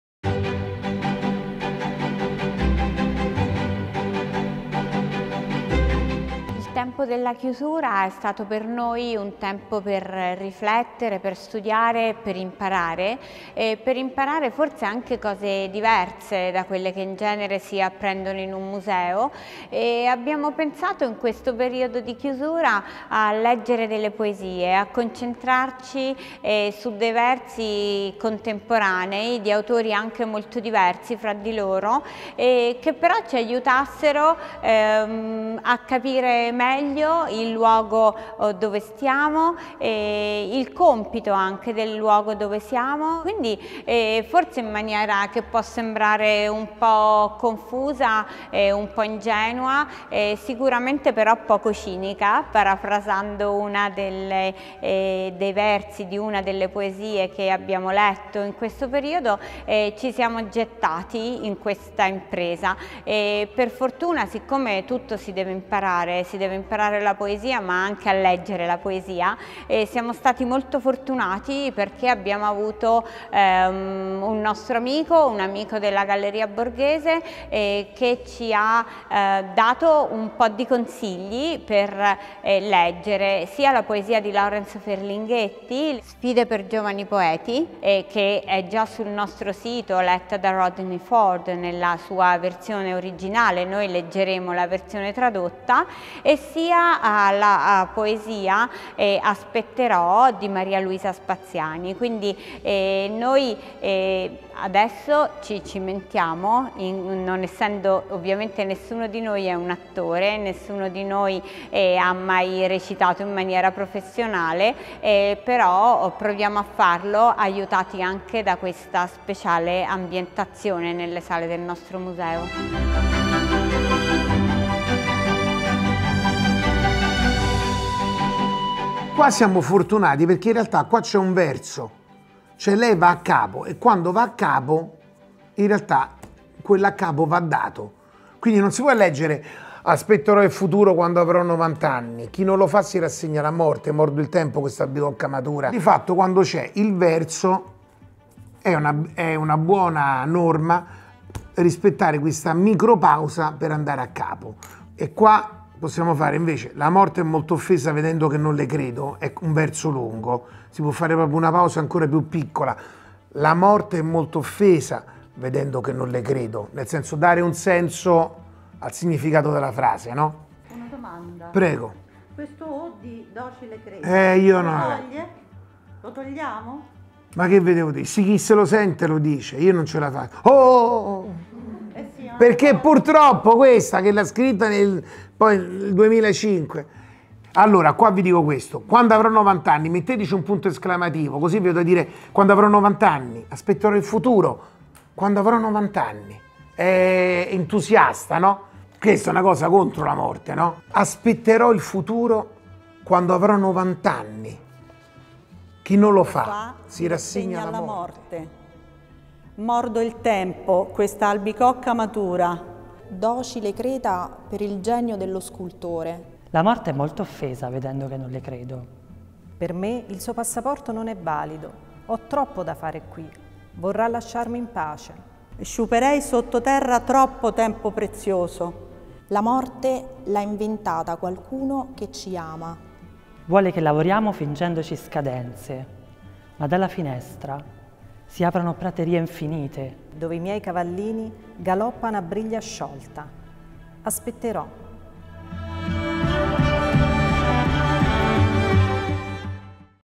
Maria Luisa Spaziani - Aspetterò
Nella giornata mondiale della poesia la Galleria Borghese presenta un nuovo format dedicato al dialogo e alla sintesi delle arti: "La Galleria Borghese ispira - Poesia al museo".

Un ringraziamento a Riccardo Rossi per i suoi preziosi consigli a supporto della lettura.

La Galleria Borghese ispira - Poesia al museo Ep.1
🖋 Maria Luisa Spaziani
📃 Aspetterò